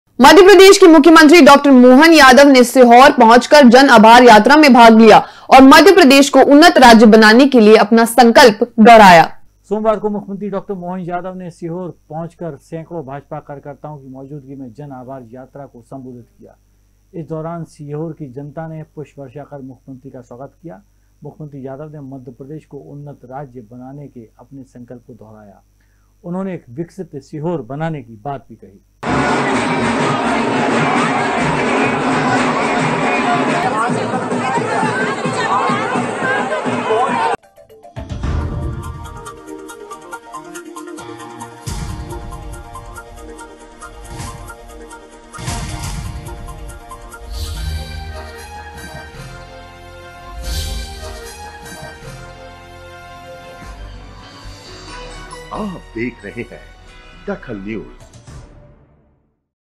DAKHAL NEWS सीएम की चुनावी जन आभार यात्रा
सीएम की चुनावी जन आभार यात्रा
पुष्प वर्षा कर सीएम का स्वागत